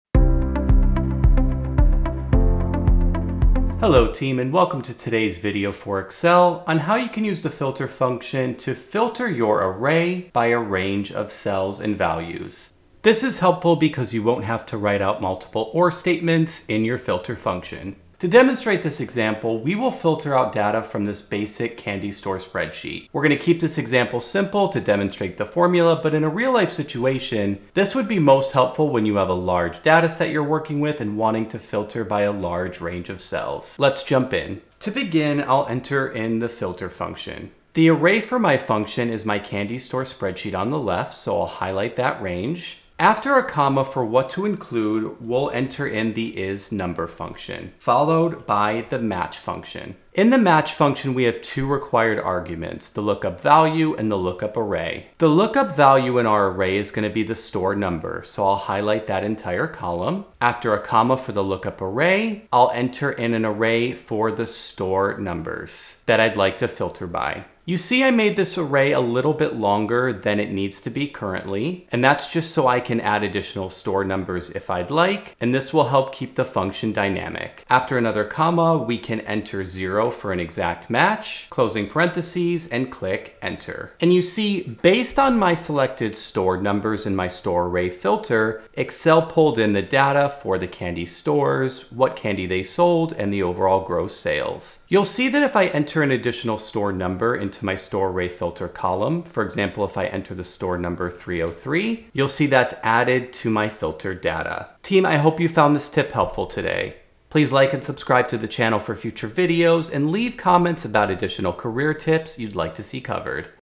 Excel Tips - FILTER Function Using Cell Range Instead of Multiple OR Statements
Hey team,

In this video we cover an Excel formula that can help you filter your data using a selected range of cells instead of needing to enter multiple OR statements into your FILTER function.

This can be very helpful for larger data sets in which you have certain requirements for the data you are looking to target.